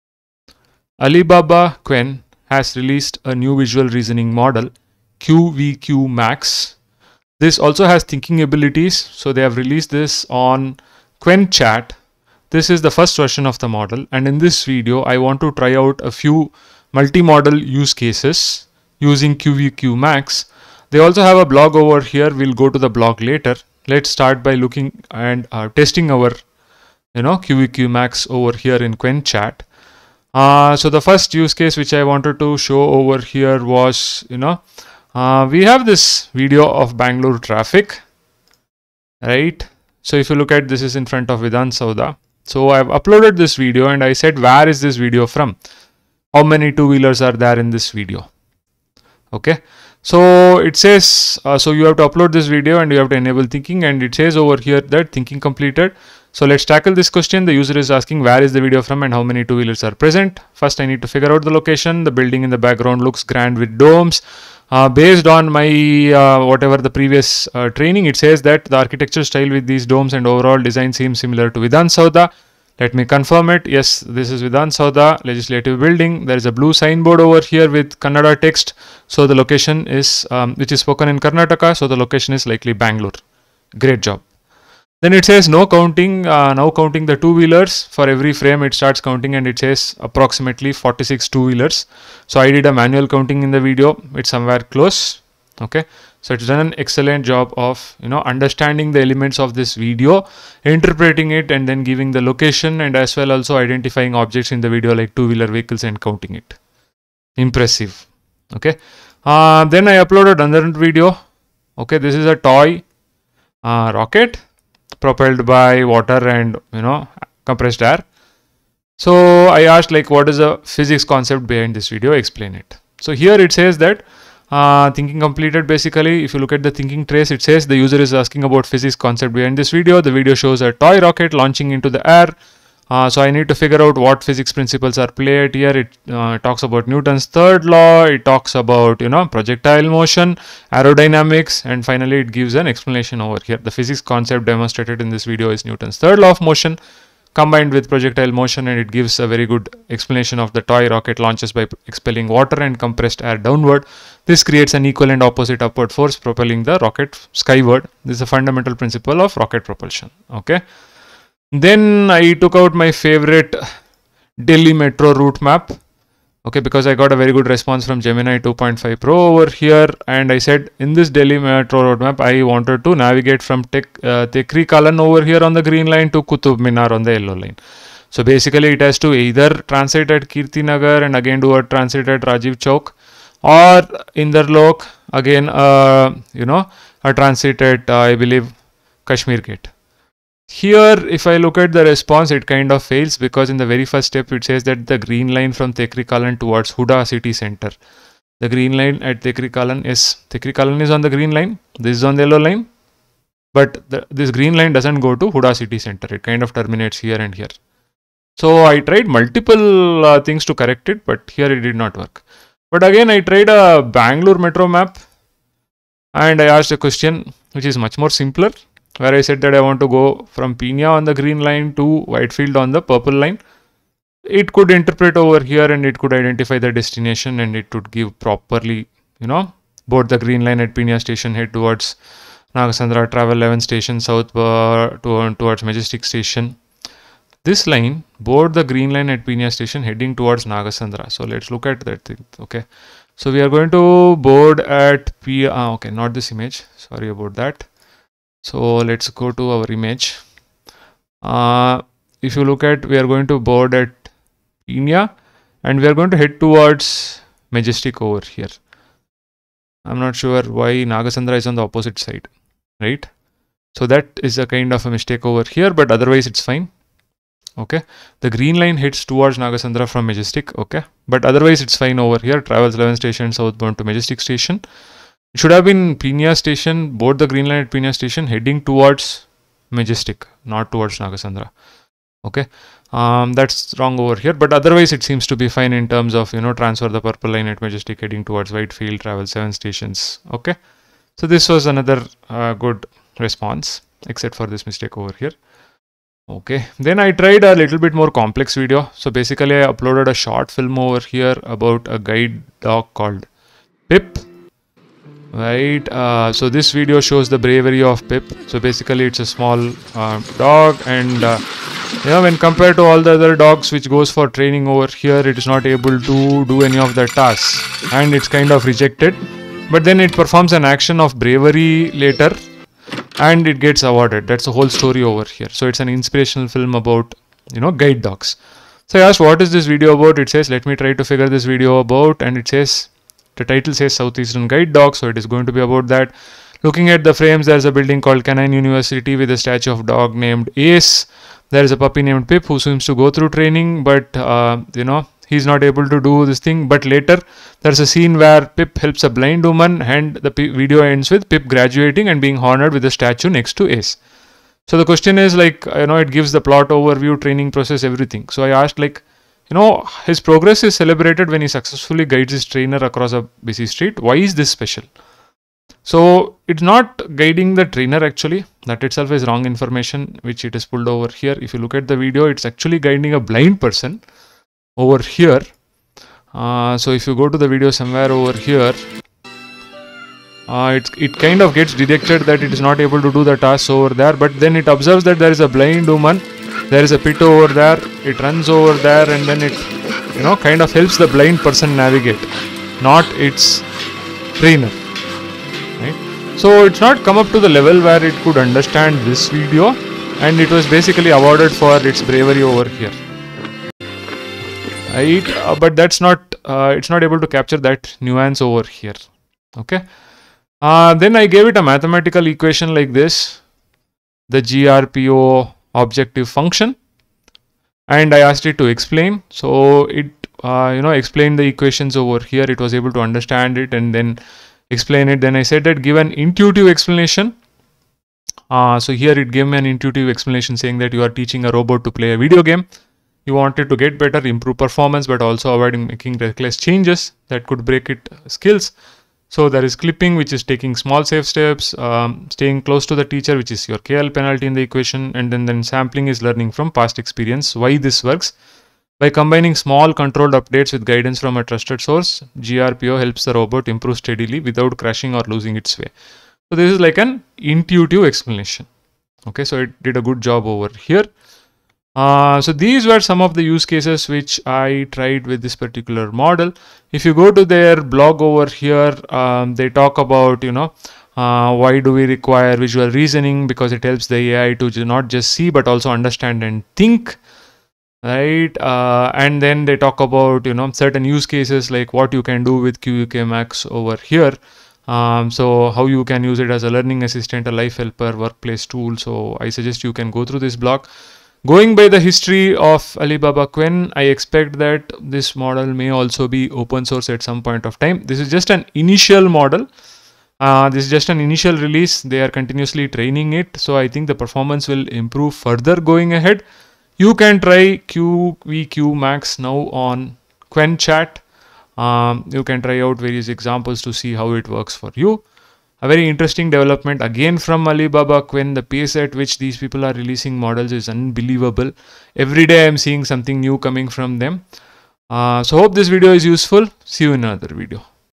Mindblowing Qwen QVQ-Max Visual AI Now Free! See What Others Cant Do! 🔥
Alibaba Qwen  just released QVQ-Max, our visual reasoning model. This model can not only “understand” the content in images and videos but also analyze and reason with this information to provide solutions. From math problems to everyday questions, from programming code to artistic creation, QVQ-Max has demonstrated impressive capabilities.

I just tested a few cases. Performance is impressive